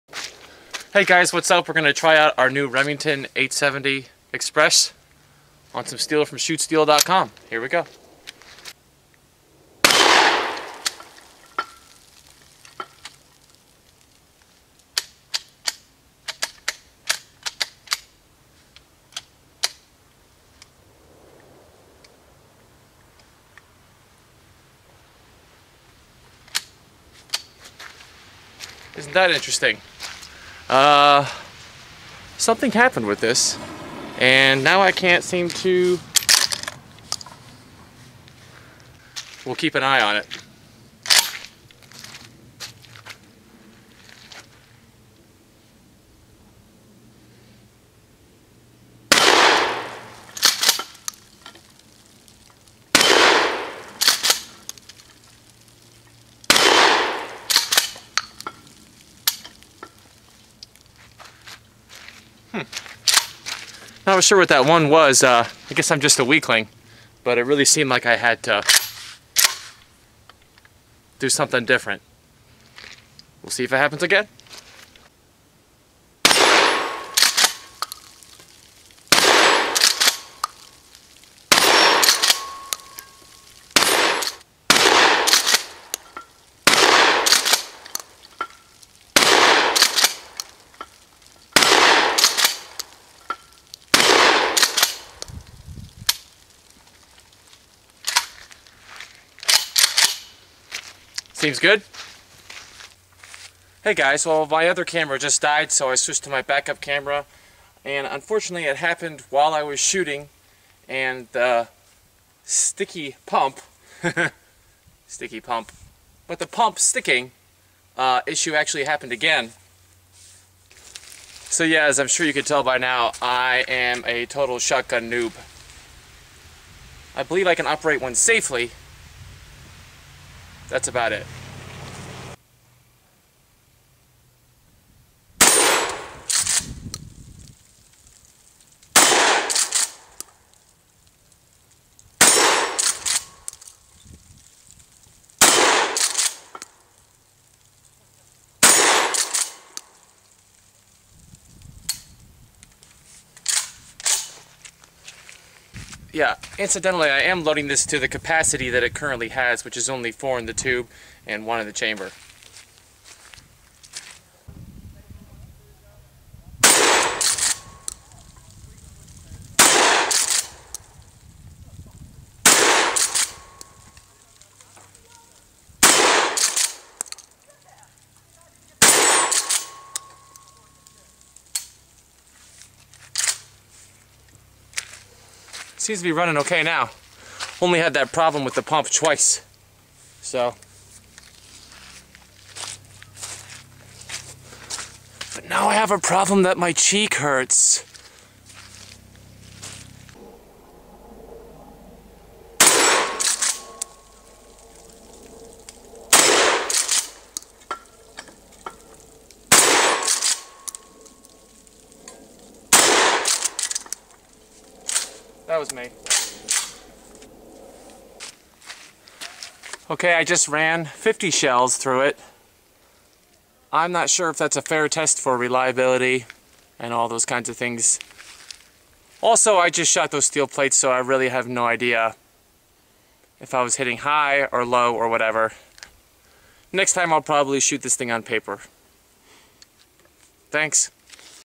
Remington 870 first shots
First time out to the range with the newly acquired Remington 870 express.  Shooting #8 Winchester birdshot target loads